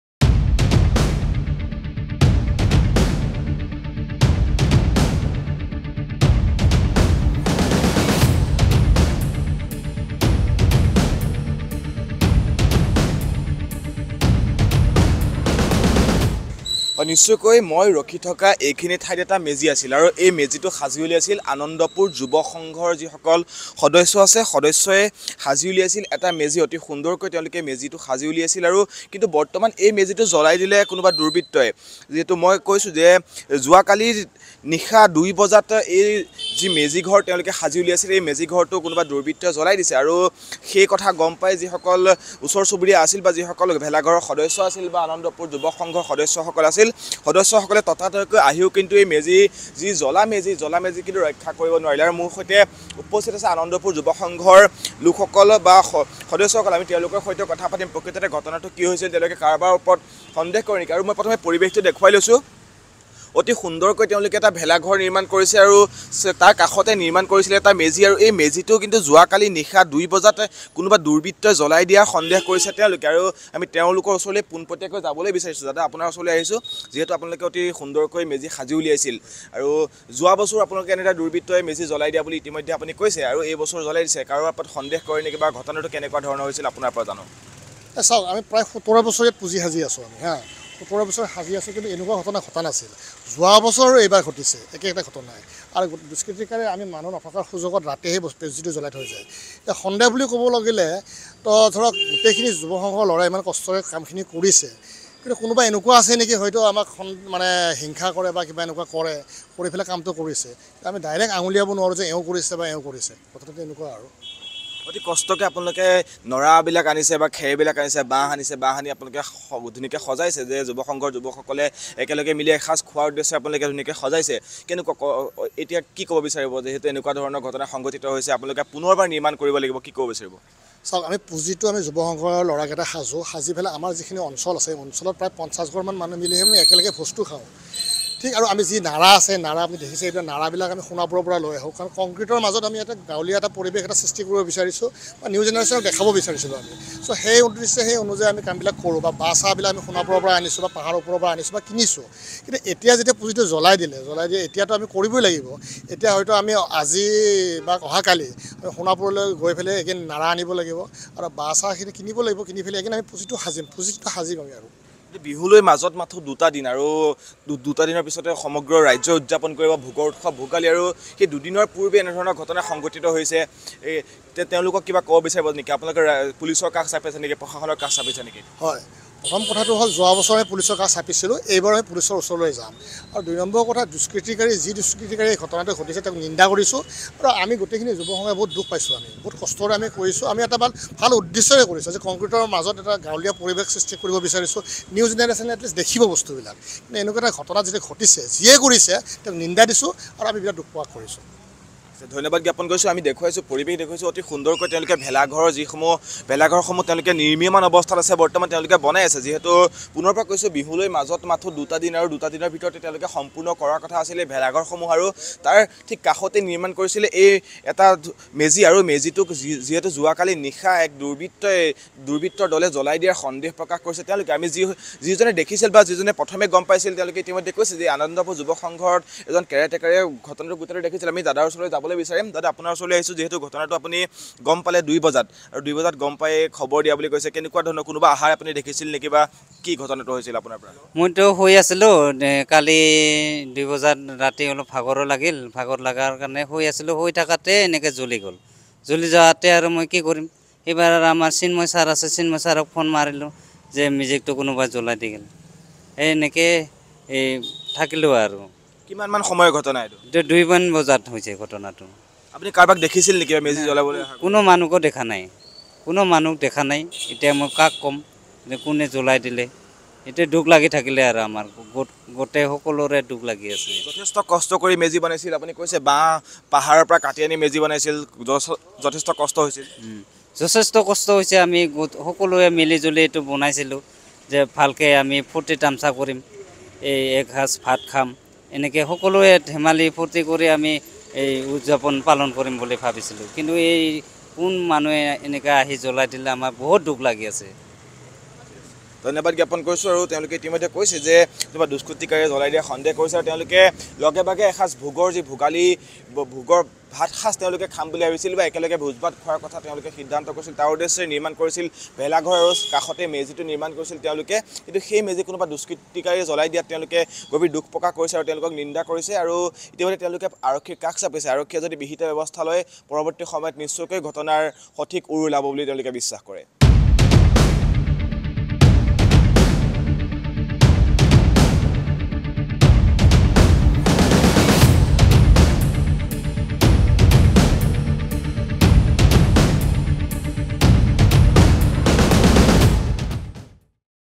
Guwahati Crime News : মহানগৰীৰ হেঙেৰাবাৰীত ভোগালী বিহুৰ পূৰ্বেই জ্বলিল মেজি। N18V
Guwahati Crime News | মহানগৰীৰ  হেঙেৰাবাৰীত ভোগালী বিহুৰ পূৰ্বেই  জ্বলিল মেজি।
আনন্দপুৰ যুৱক  সংঘৰ  উদ্যোগত   শ্ৰী শ্ৰী  লক্ষ্মী মন্দিৰ  প্ৰাংগনত    ভোগালী বিহু উদযাপন কৰাৰ চলাইছিল  প্ৰস্তুতি।
নিশা কোনো দুৰ্বৃত্তই  মেজিত অগ্নি সংযোগ কৰাৰ সন্দেহ আনন্দ পুৰ যুৱক সংঘৰ সদস্যৰ
যোৱা বছৰো একেদৰে দুস্কৃতি কাৰীয়ে জ্বলাইছিল মেজি
এনেদৰে ঘটনাৰ পুনৰাবৃত্তি ঘটাক লৈ চঞ্চল্য সৃষ্টি কৰিছে অঞ্চলটোত
ইতিমধ্যে যুৱক সমিতিৰ সদস্য সকলে কাষ চাপিছে আৰক্ষী প্ৰশাসনৰ